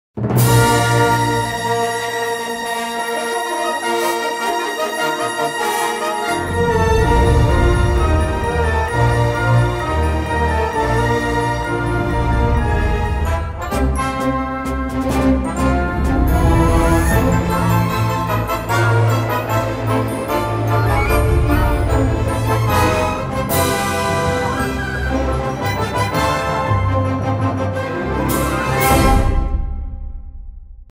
John Williams Tour Trailer
The Best of John Williams with the orchestra of the Minsk Bolshoi Theater conducted by Claudio Vandelli